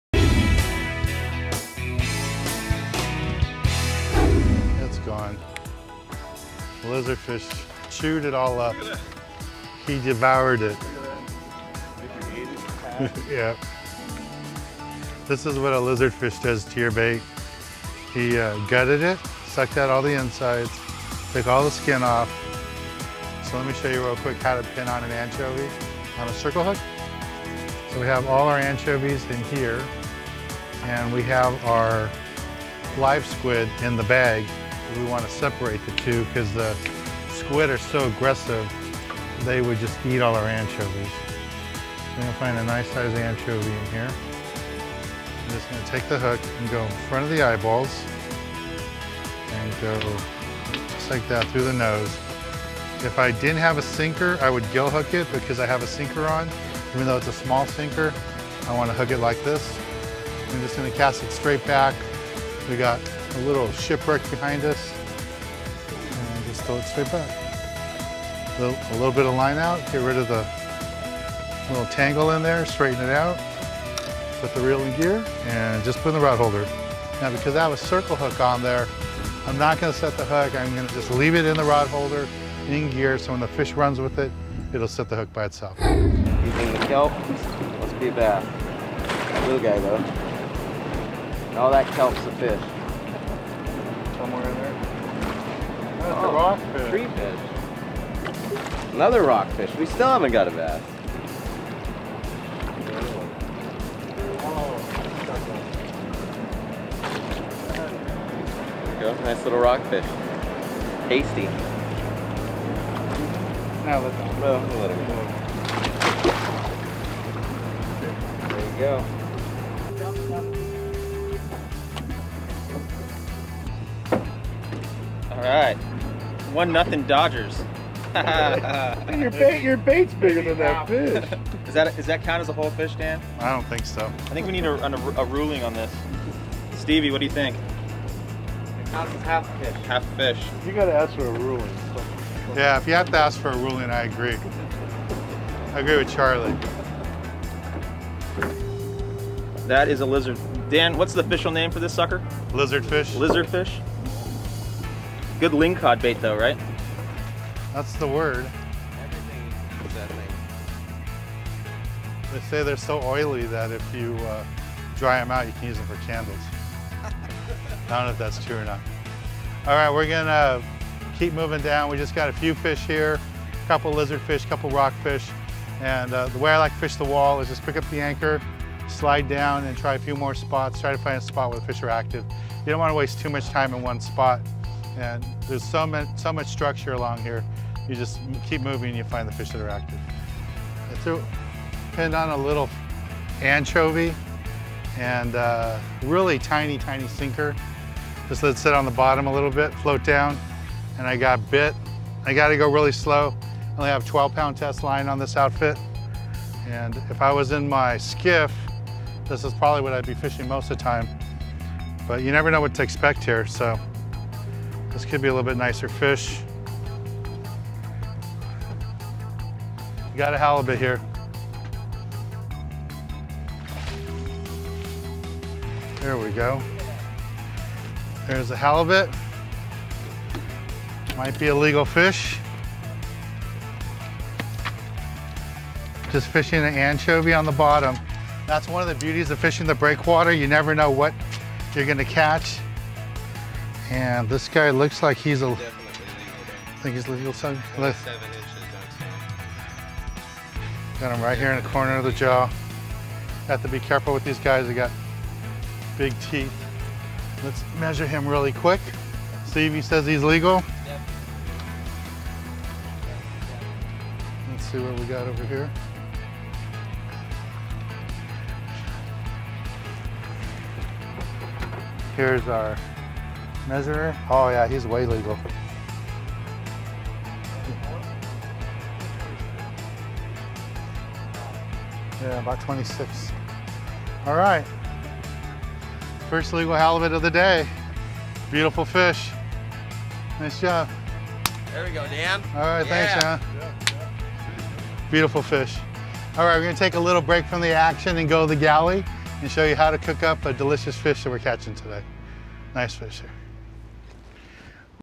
Halibut On The Wall | SPORT FISHING
Dan takes some friends fishing to the breakwater and lands a legal size halibut. Use this link to see Dan fishing charters,